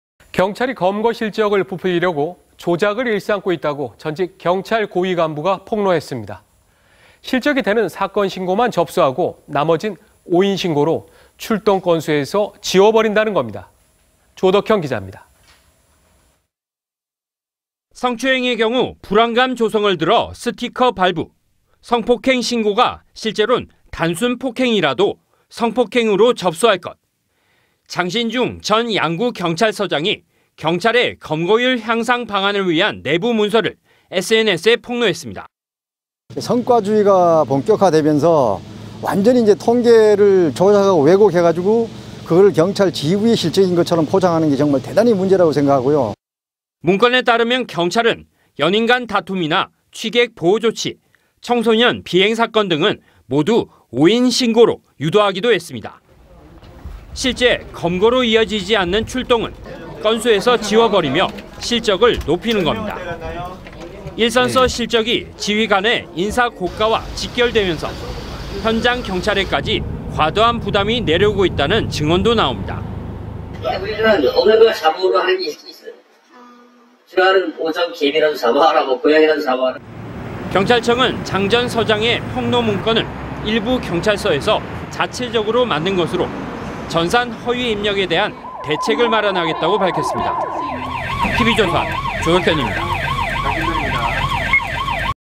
前경찰 고위간부 "경찰, 검거율 통계 조작"
경찰이 검거 실적을 부풀리려고 조작을 일삼고 있다고 전직 경찰 고위 간부가 폭로했습니다. 실적이 되는 사건 신고만 접수하고 나머진 '오인' 신고로 출동 건수에서 지워버린다는 겁니다.